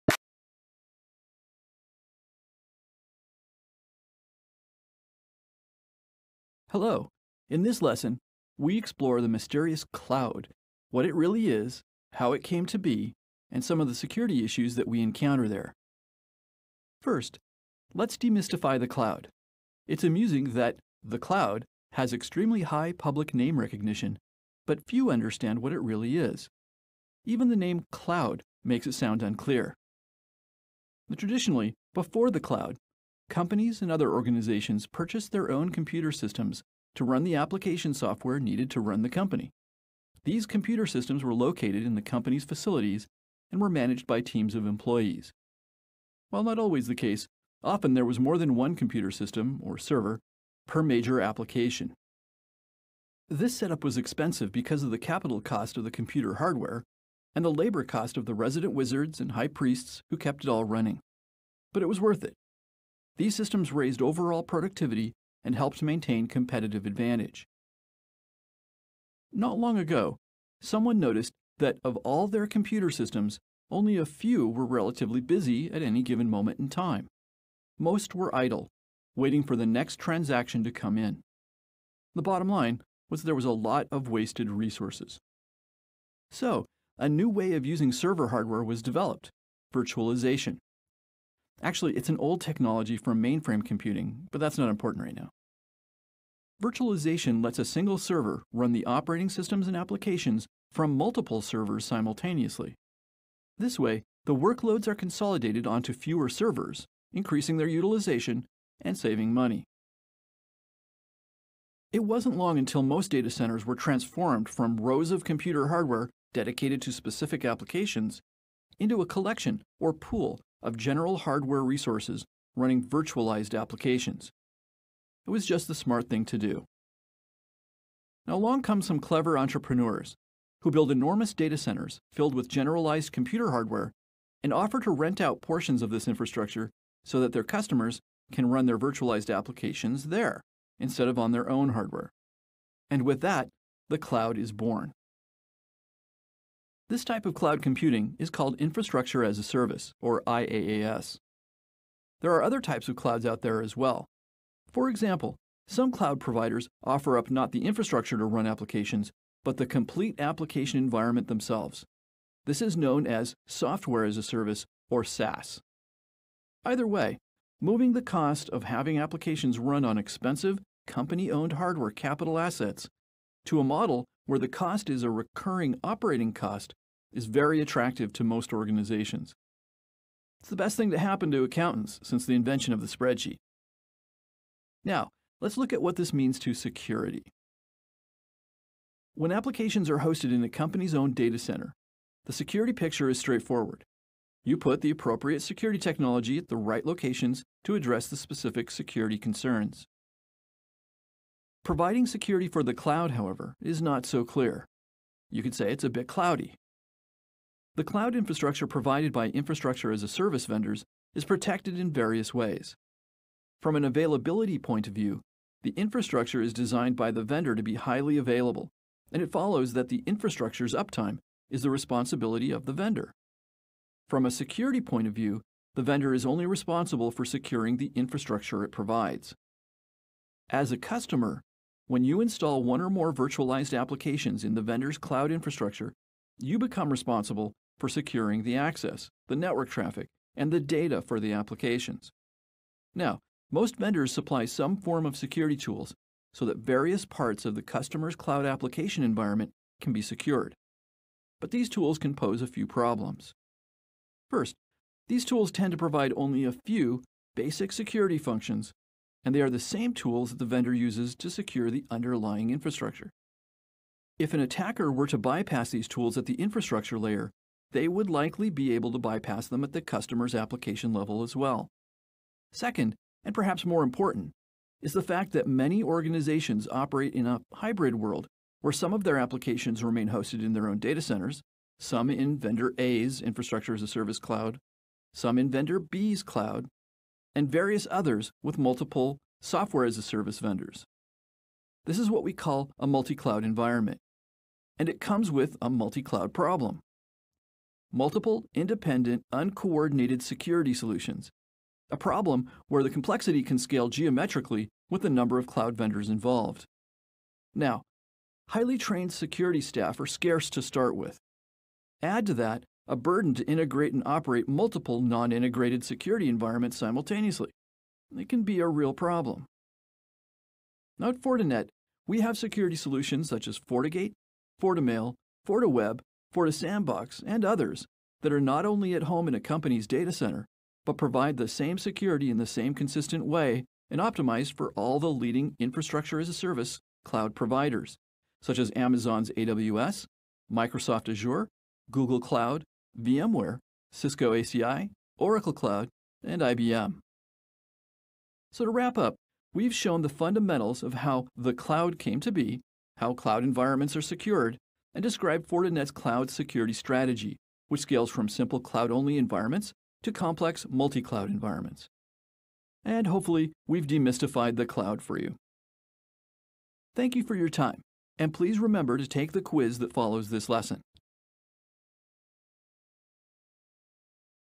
NSE 2 - Cloud Security
In this lesson, we explore the mysterious "cloud", what it really is, how it came to be, and some of the security issues that we encounter there.